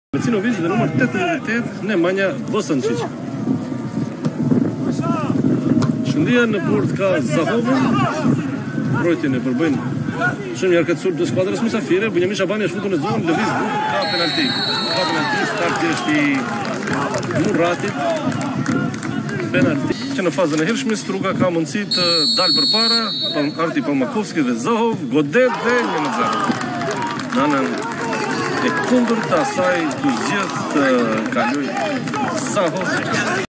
ПМФЛ - 31. коло (ШКЕНДИЈА - СТРУГА ТЛ 0-1) Гол на Алтипармаковски од пенал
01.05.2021 - ПМФЛ - 31. коло (ШКЕНДИЈА - СТРУГА ТЛ 0-1) Гол на Алтипармаковски од пенал